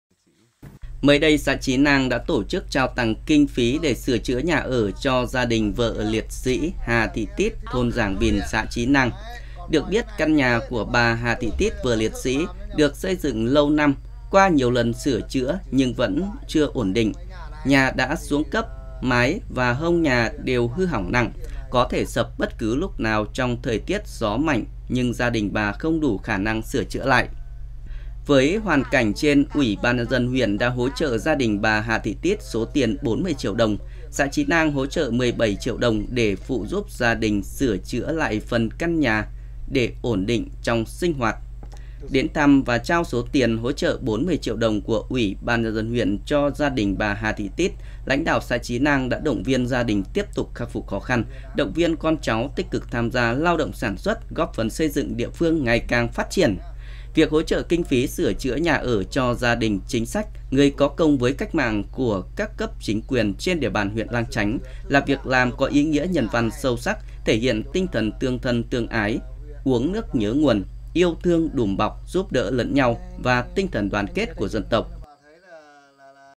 Hỗ trợ kinh phí sửa nhà cho gia đình chính sách
Mới đây, xã Trí Nang đã tổ chức trao tặng kinh phí để sửa chữa nhà ở cho gia đình vợ liệt sỹ Hà Thị Tít,Thôn Giàng Vìn xã trí Nang.